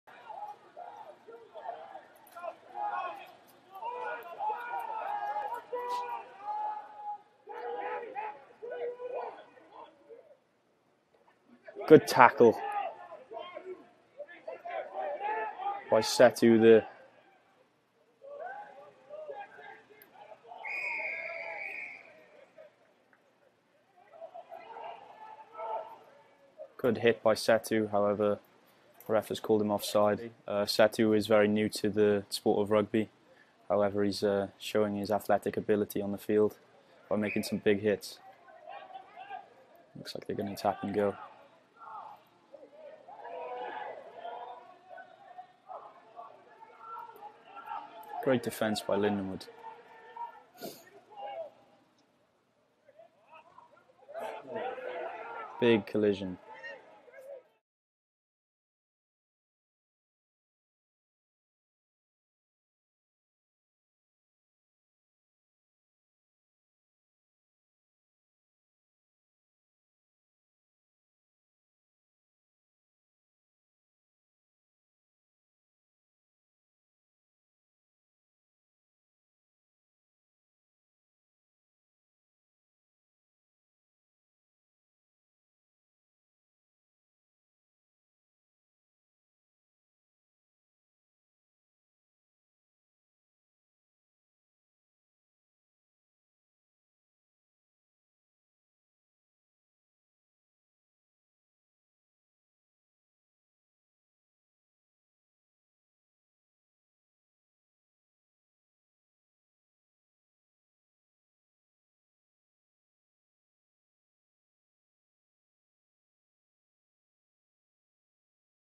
Setu Vole - Prop - Lindenwood University
Some of Setu Vole’s 2019-20 Rugby highlights at Lindenwood University.